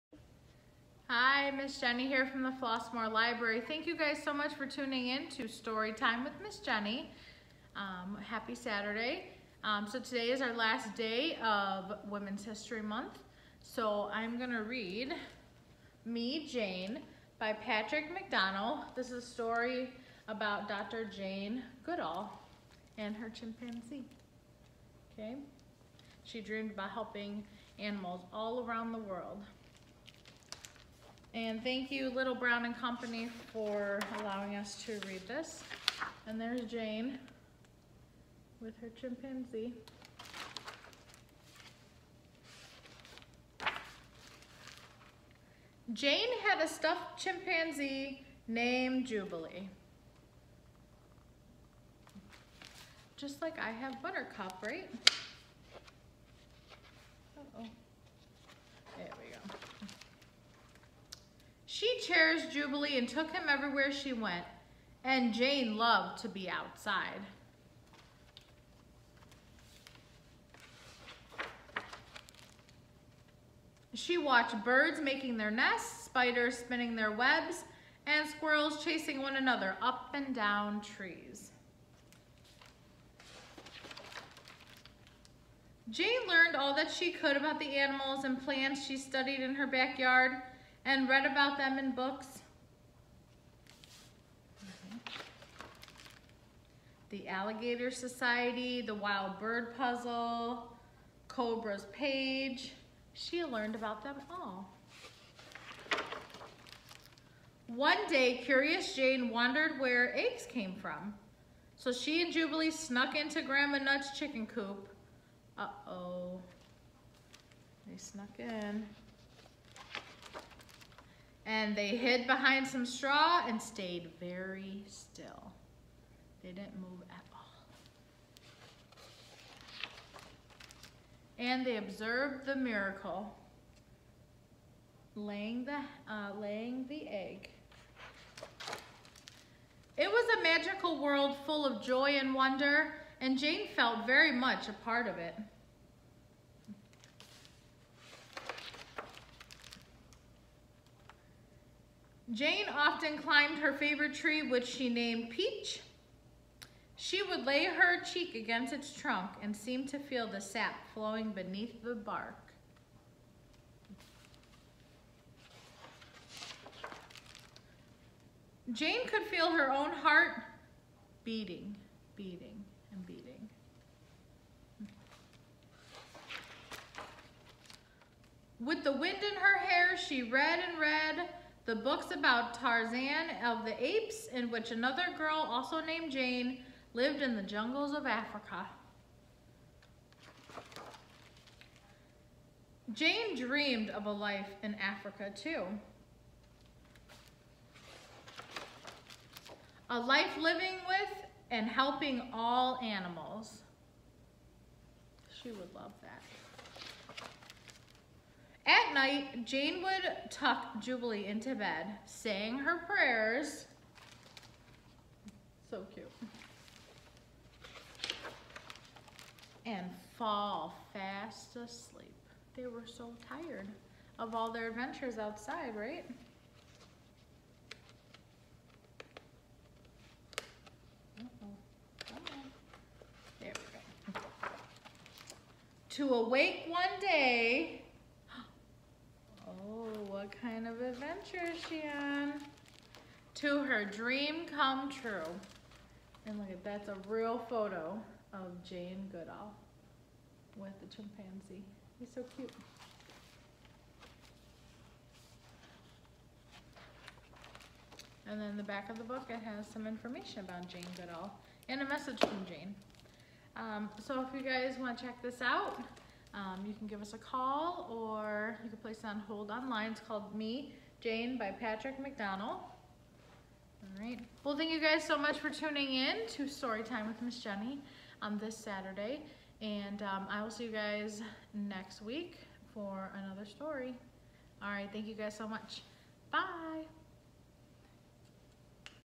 Storytime with Miss Jenny 03/27/2021
Miss Jenny from the Flossmoor Public Library reads Me. Jane by Patrick McDonnell